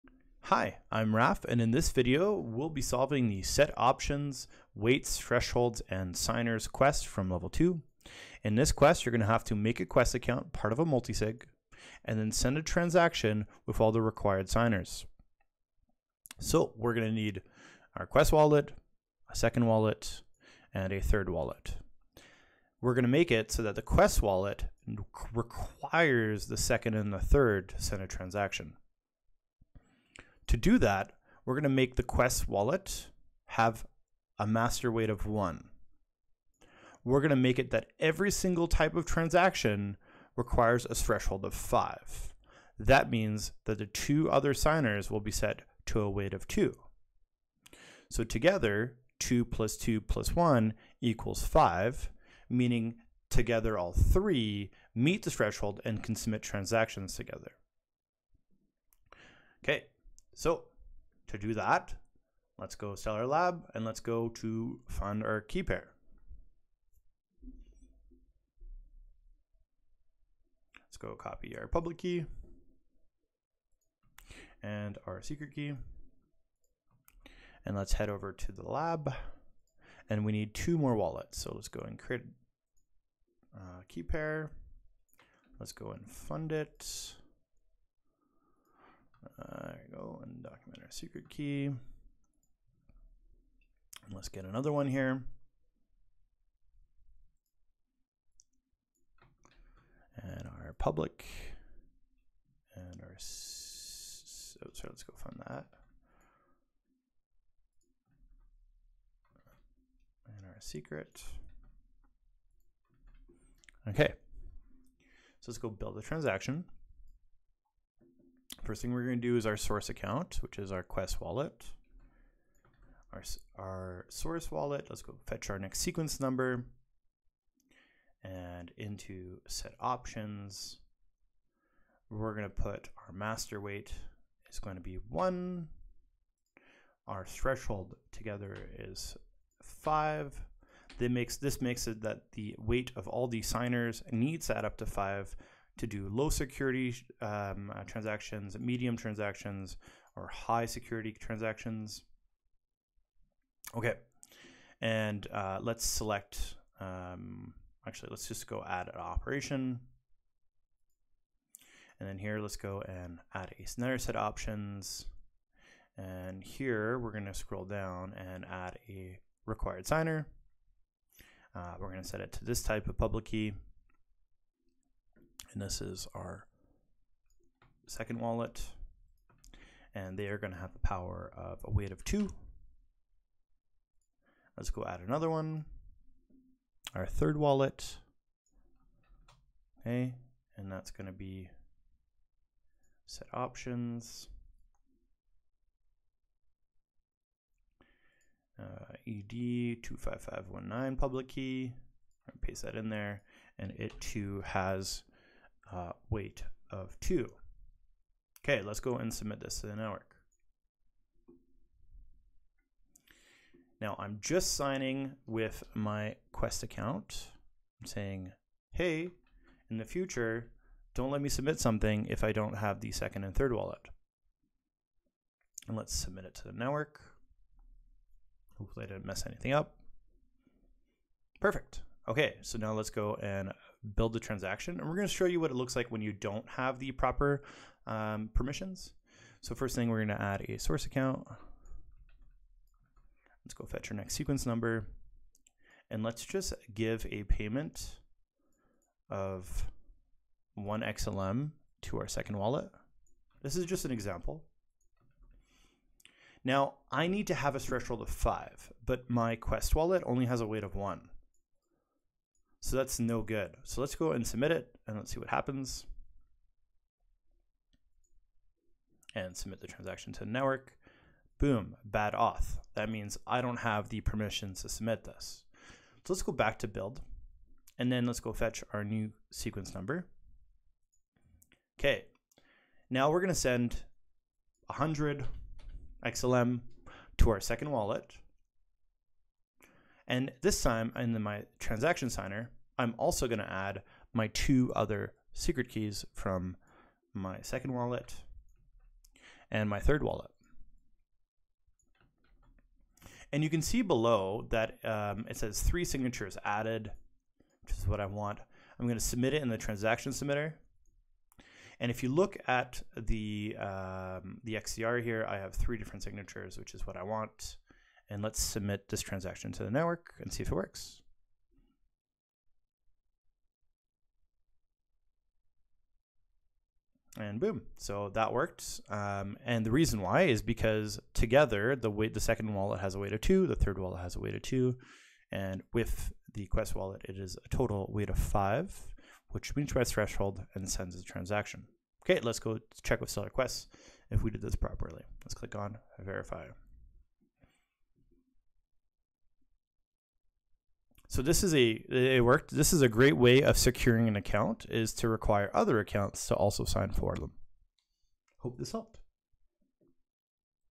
Stellar Quest Level 2 - Set Options Weights, Thresholds, and Signers
Tutorial video helping you solve the level 2 activity "Set Options - Weights, Thresholds, and Signers" from Stellar Quest.
Stellar Quest is a series of challenges helping you learn how to use and develop for the Stellar blockchain.
Questions about this quest? Head over to the Stellar Quest Discord